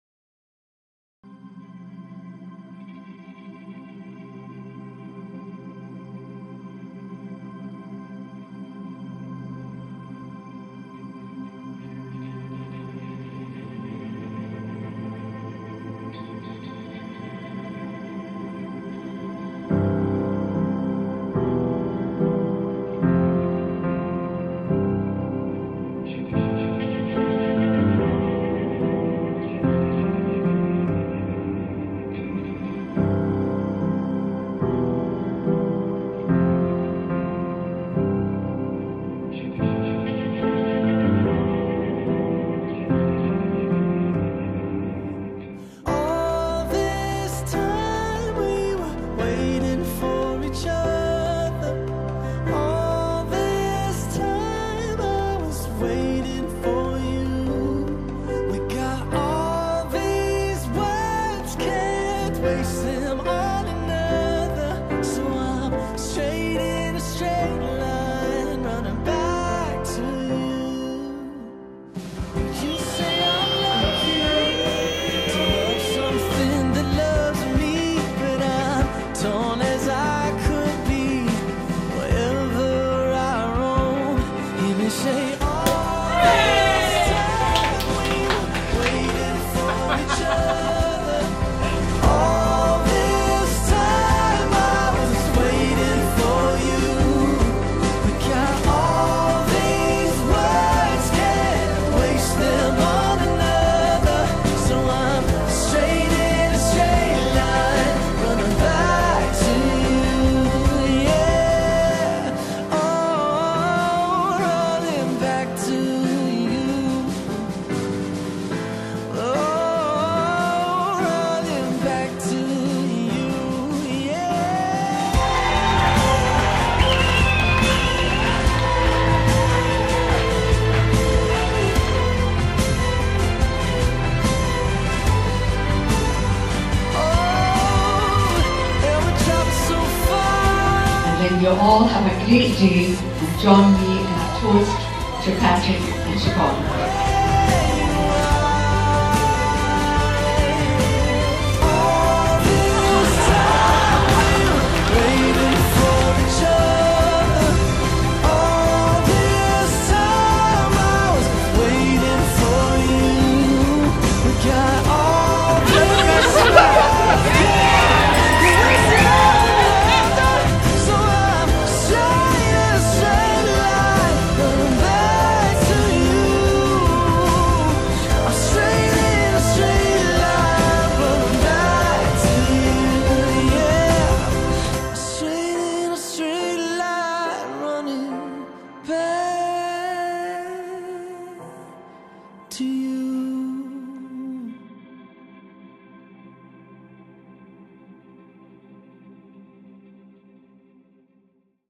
Siobhan & Pat's Wedding
My beautiful cousin, Siobhan, and her husband were married this past summer. It was a beautiful day in Gort, with after drinks in Hennelly's Bar and an amazing reception in Lady Gregory Hotel